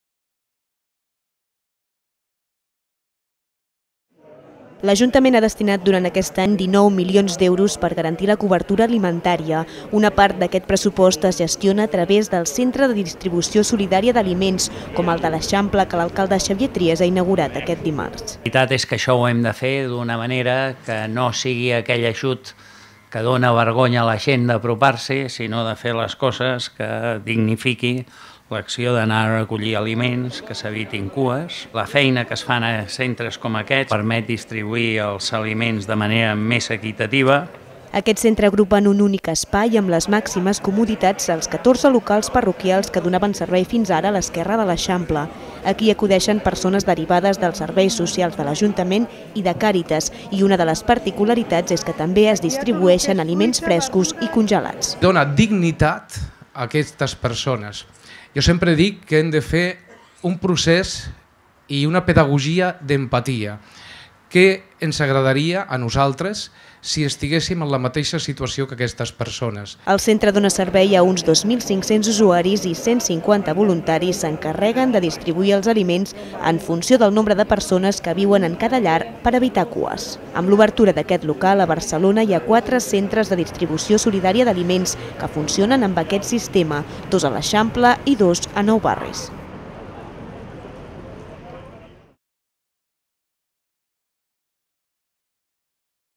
Barcelona compta amb un nou Centre de Distribució Solidària d'Aliments
L’Ajuntament ha destinat durant aquest any 19 milions d’euros per garantir la cobertura alimentària. Una part d’aquest pressupost es gestiona a través dels Centre de Distribució Solidària d’Aliments, com el de l’Eixample que l’alcalde Xavier Trias ha inaugurat aquest dimarts.